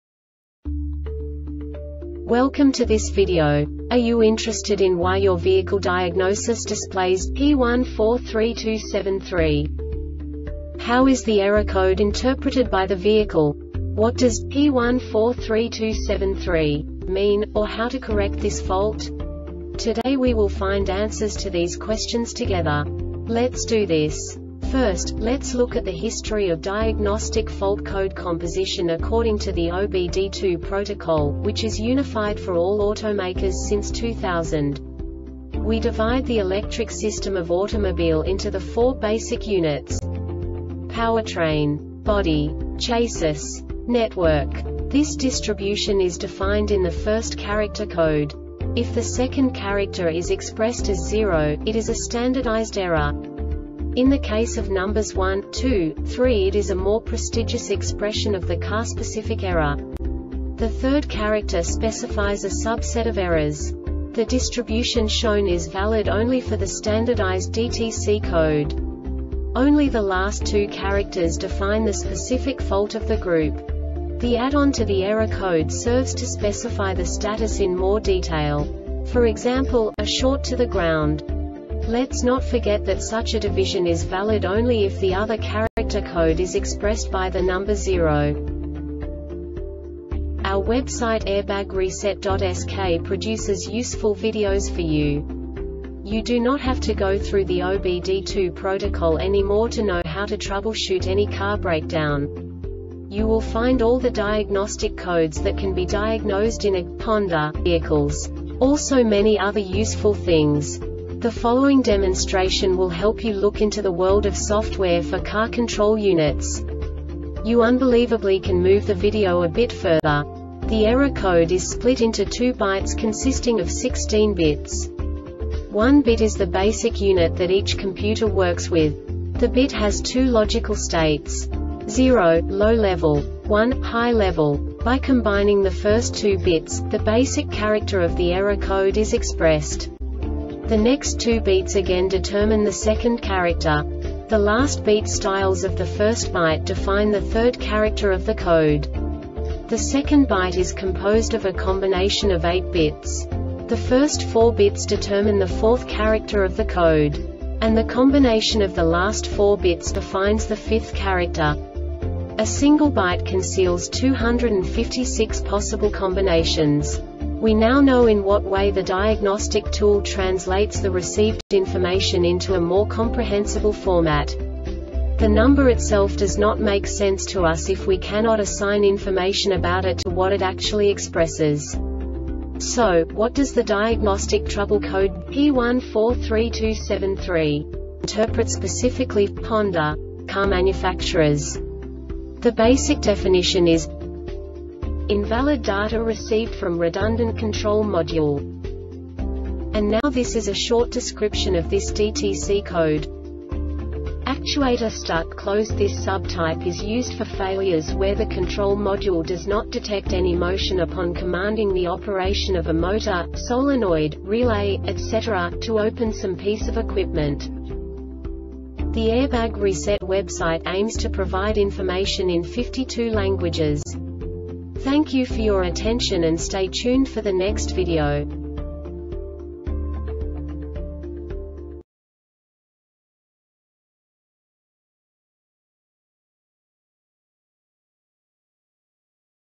DTC Honda P1432-73 Short Explanation
Contents:
0:21 Basic DTC analysis according to OBD2 protocol standard.
1:48 Insight into programming
2:58 A diagnostically understandable description of the Diagnostic Trouble Code
3:05 Basic error definition

Make:
Honda
Code:
P1432 73
Definition:
Invalid Data Recieved From Redundant Control Module
Failure Type: